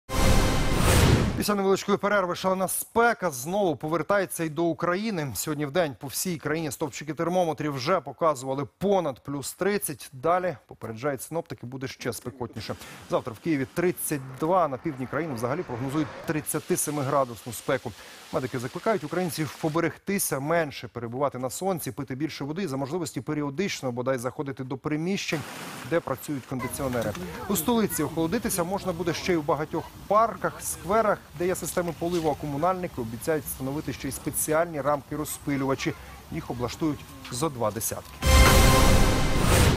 В Україну знову повертається шалена спека
Сьогодні вдень по всій країні стовпчики термометрів вже показували понад +30. Далі, попереджають синоптики, буде ще спекотніше. Завтра в Києві 32, а на півдні країни взагалі прогнозують 37-ми градусну спеку.